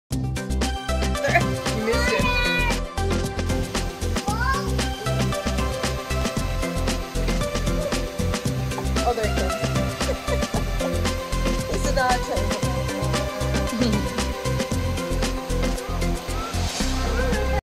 Our Immunocompromised Toddler's First Time Meeting RED PANDA
Join us in our travel vlog as we share our first family trip to the Washington DC Zoo with our 3-year-old immunosuppressed toddler, Celeste Anne, who is a kidney transplant survivor. Despite our initial worries about traveling with an immunocompromised child, we were able to make it happen with proper precautions and planning. Our 16-year-old son, Harp, also had a great time. Watch as we explore the zoo, meet a red panda, and experience the joys of traveling as a family. This inspiring story showcases our journey of resilience and how we overcame the challenges of kidney failure to create wonderful memories. Don't miss our tips for safe travel with an immunocompromised child.